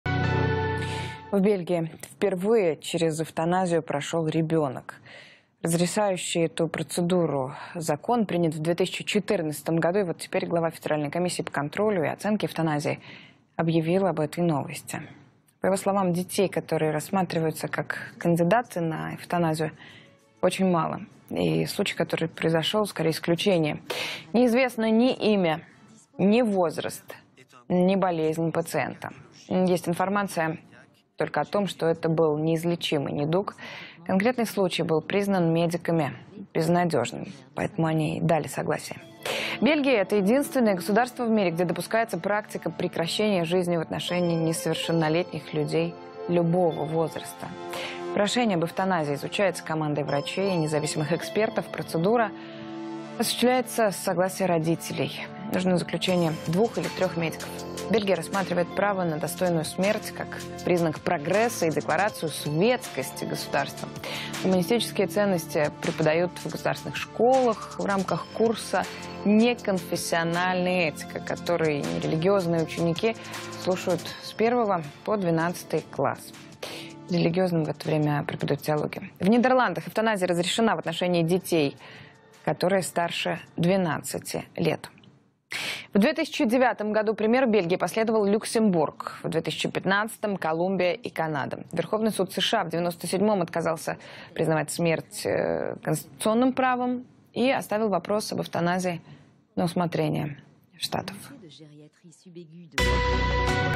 В Бельгии подвергли эвтаназии несовершеннолетнего
Эвтаназия людей всех возрастов разрешена лишь в Бельгии